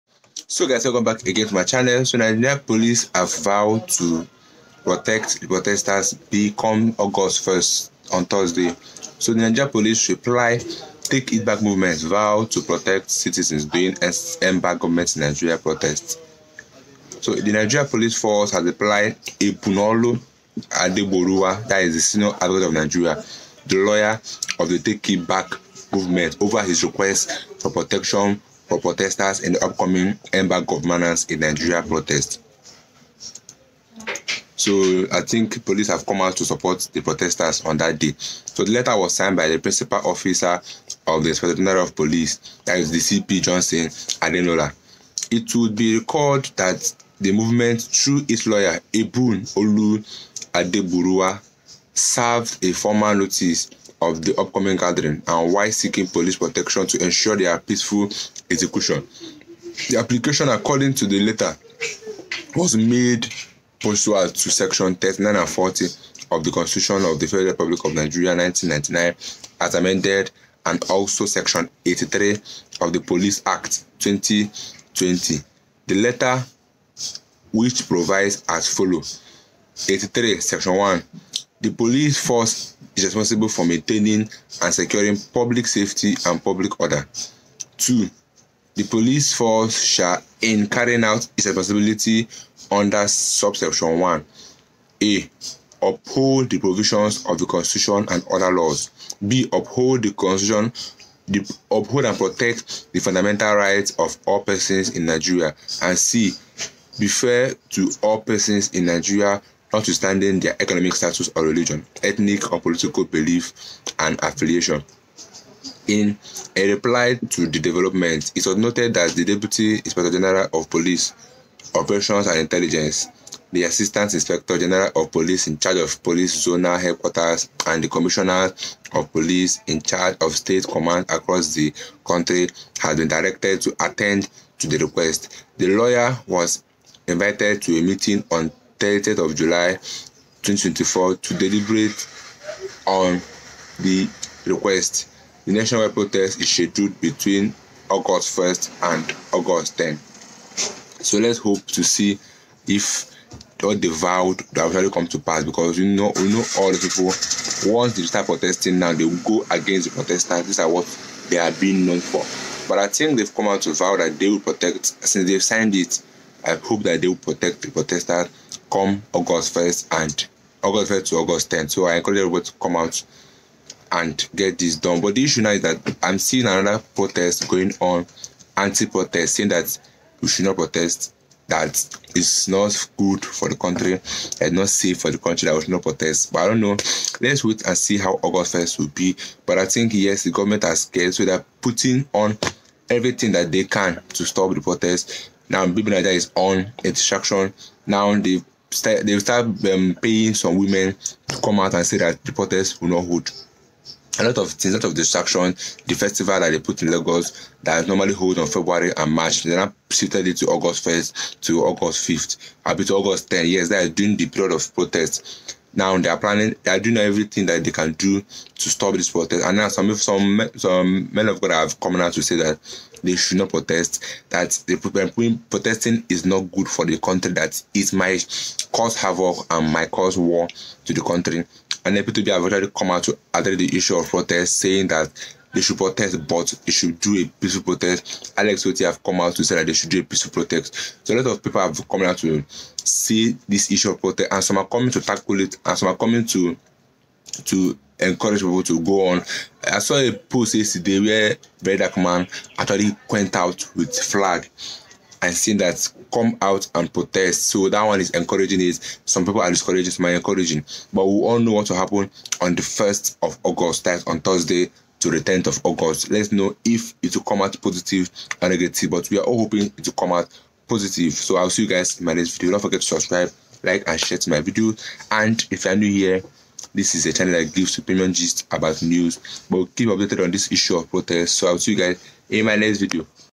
Breaking News; Tinubu Panics as Plice bows to pressure EndbadgovernanceinNigeria On The 1 August
SAN Urges police to ensure the protection of protesters on the coming Protest